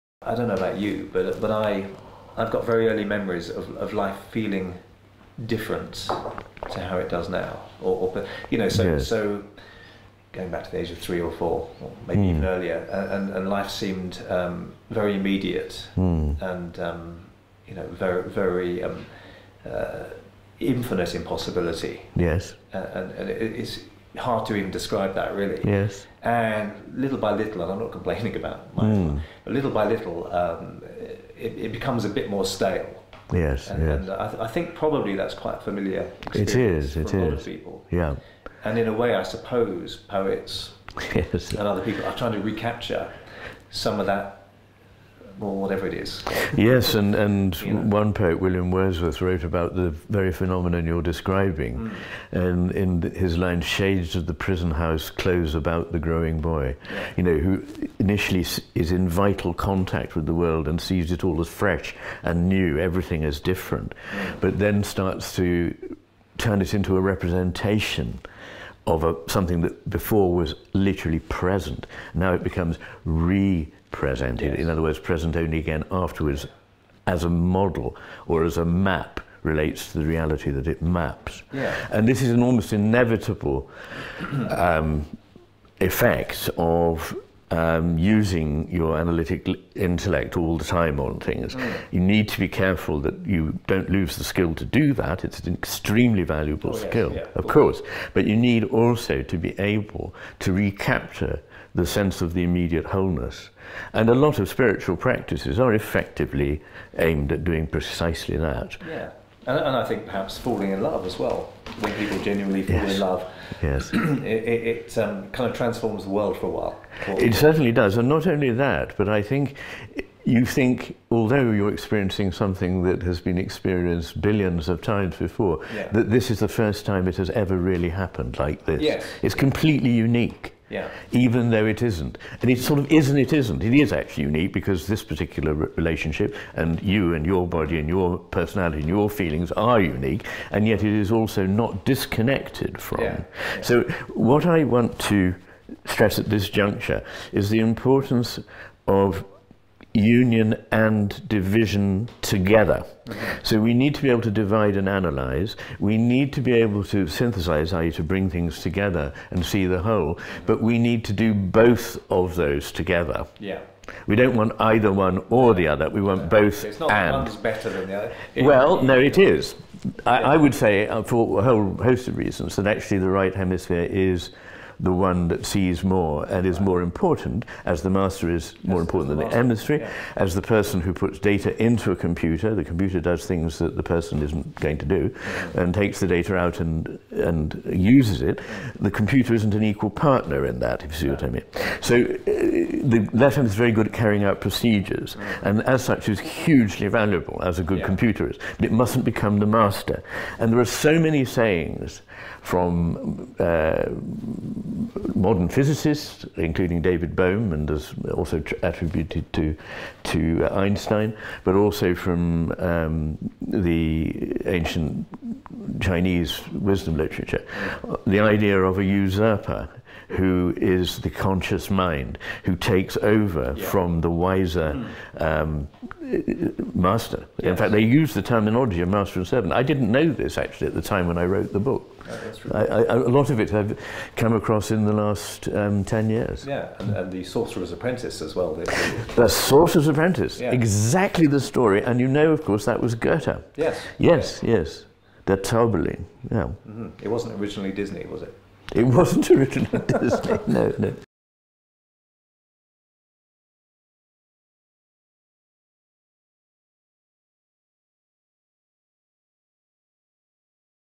The Dangers of Relying on Your Analytical Intellect Too Much
Dr McGilchrist discusses the effects of overusing analytical intellect on our view of the world.